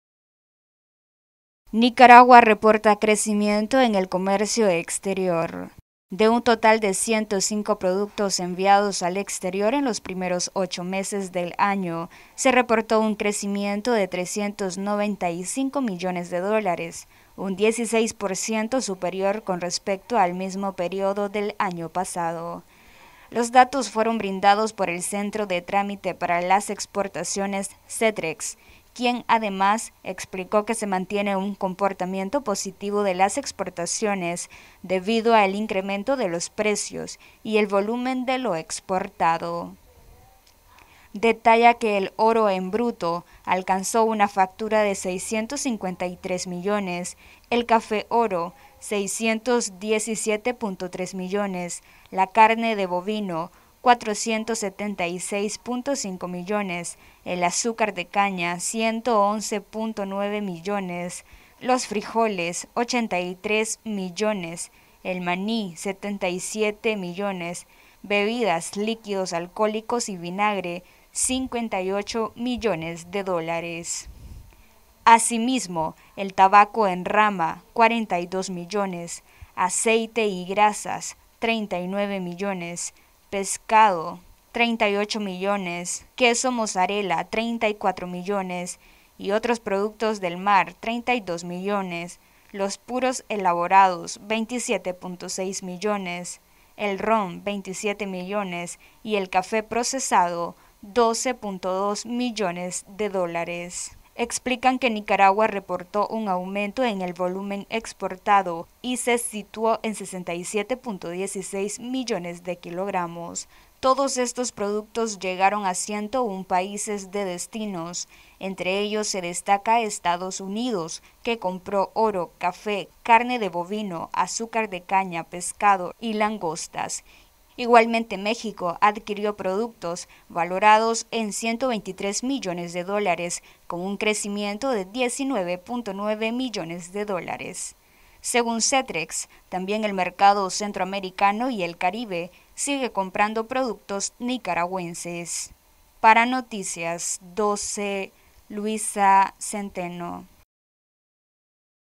Nicaragua reporta crecimiento del 16% en comercio exterior
Noticias12Nicaragua Nicaragua reporta crecimiento del 16% en comercio exterior, según datos brindados por el Centro de Trámite para las Exportaciones, CETREX.